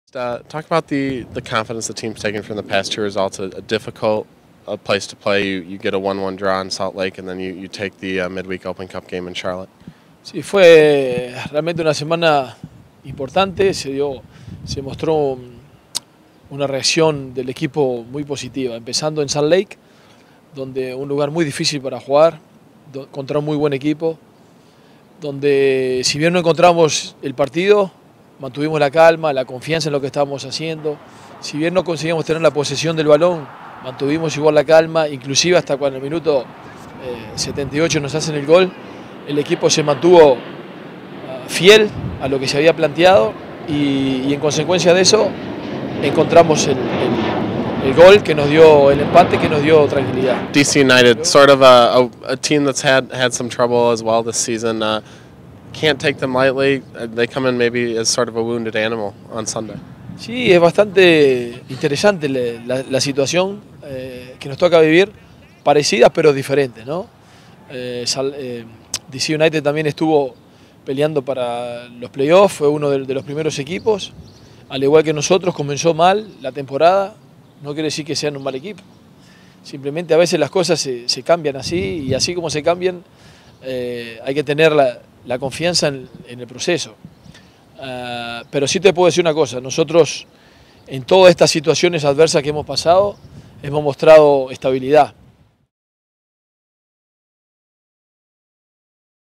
Leo Percovich Habla sobre DC United
Leo Percovich Habla sobre DC United y el gane en U.S Open Cup